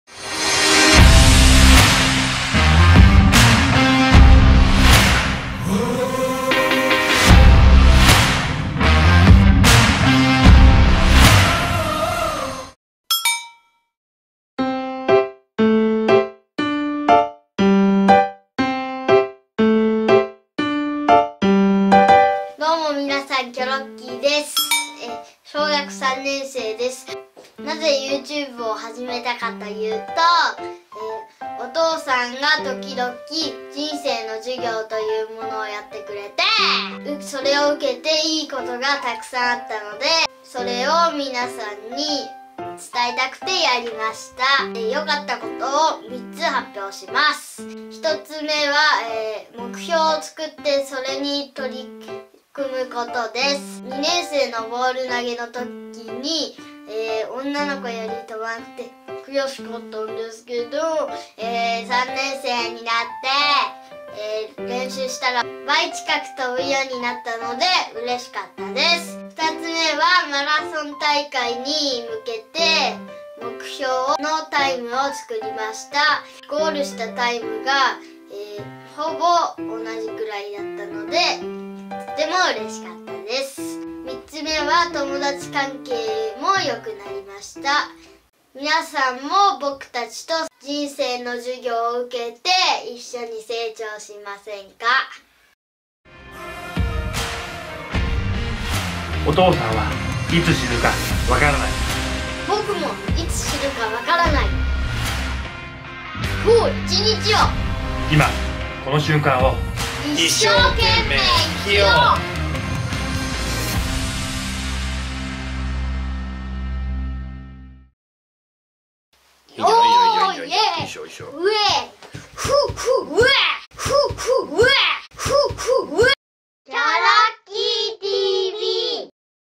親子で学ぶ！学校では教えてくれない「人生の授業」【予告編】
新レーベル「キョロッキーTV」始動！！

小学校３年生のキョロッキーとその父親が、
"どうしたら後悔しない人生を送れるか"を学ぶ
学校では教えてくれない「人生の授業」。

間もなく開講！乞うご期待！